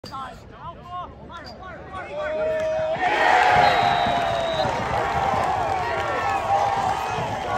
Charlie Lowe scores in the fourth quarter for Chatham vs. Randolph
Charlie Lowe scores off a move from behind the net for Chatham in the fourth quarter of the state sectional semifinal vs. Randolph